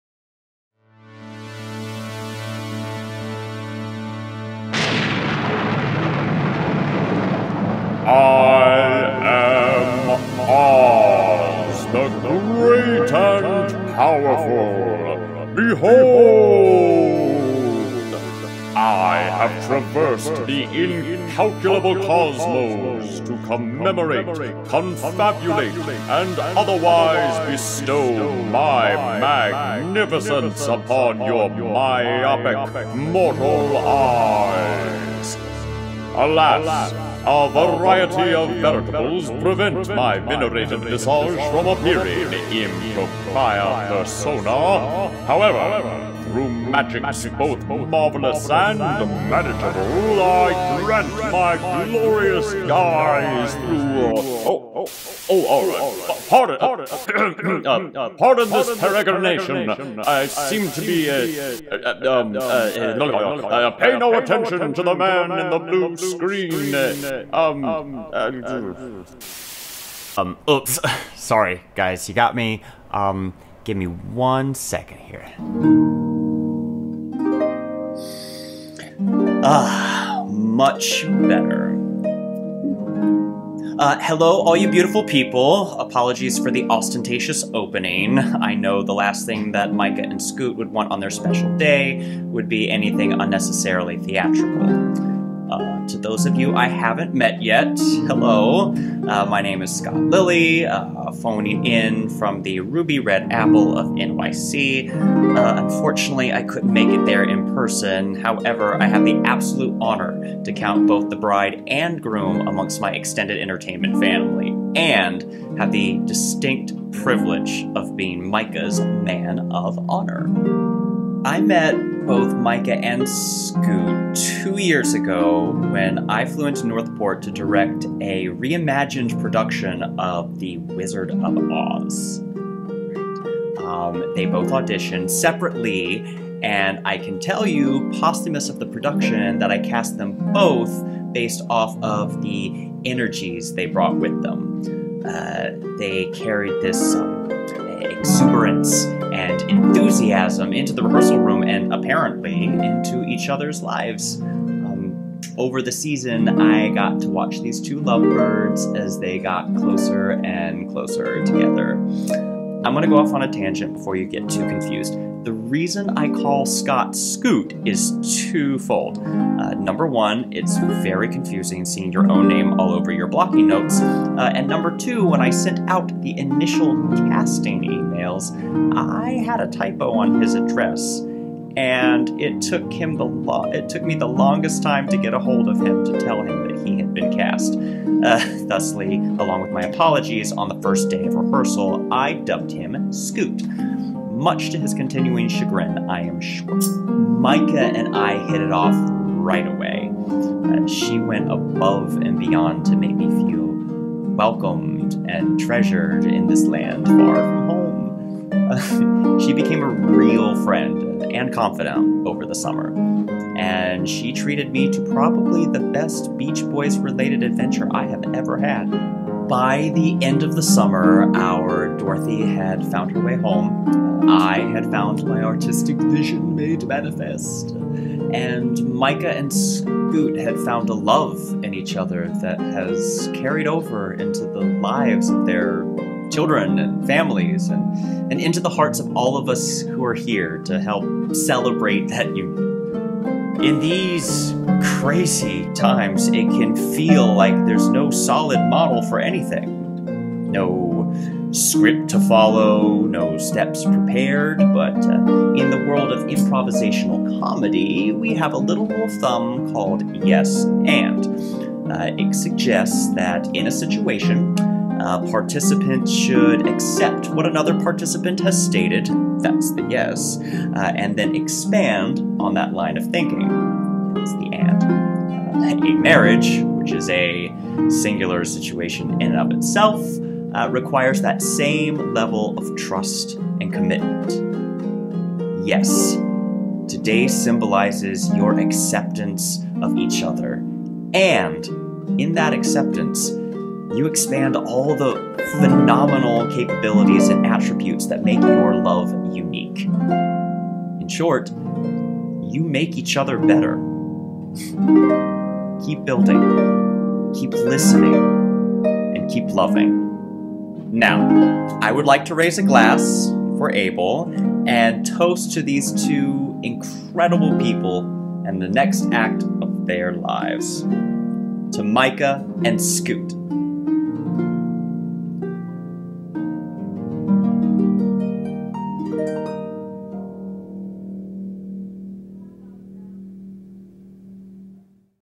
Micha & Scott Myers - OZ Wedding Toast
Micha Bancroft and Scott Myers married November, 7th 2020.  Unfortunately  I couldn't make in person to perform my MoH duties - so I put together this video to toast the newly nuptial'd couple - may their Love take them Over the Rainbow...